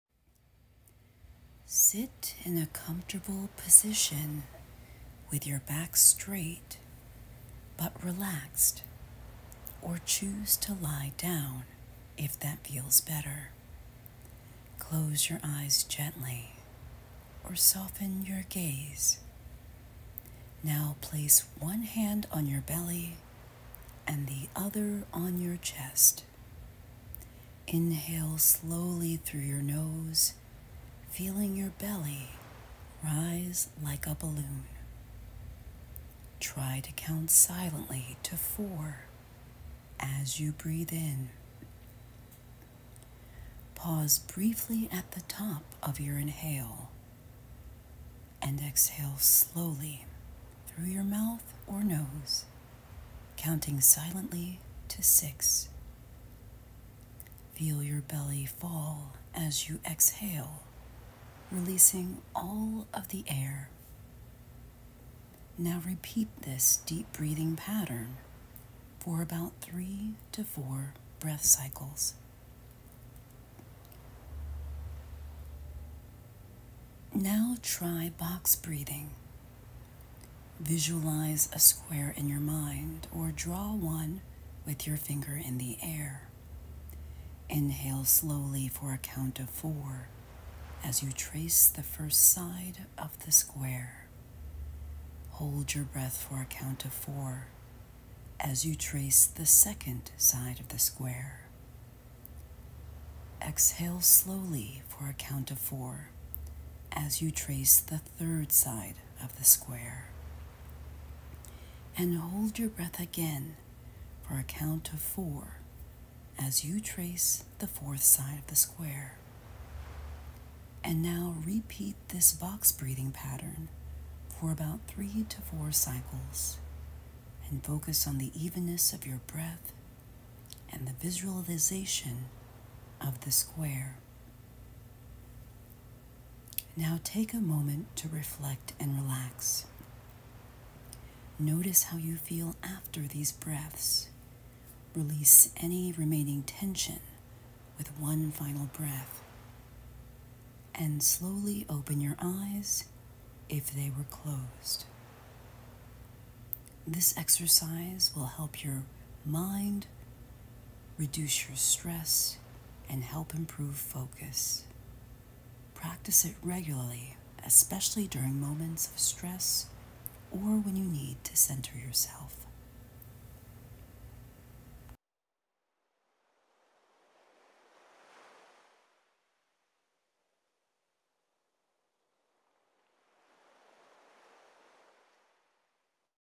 🧘‍♂️ Breathing Exercises for RELAXATION 🧘‍♂️
Do you need to relax? These breathing exercises will help you relax! Check out this video, as this video will help you!

This video is short and you can do this meditation anytime and anywhere. Repeat it throughout the day to help center yourself and focus on confidence!

Follow for more mindfulness meditation videos to help you live a healthy, fulfilled life!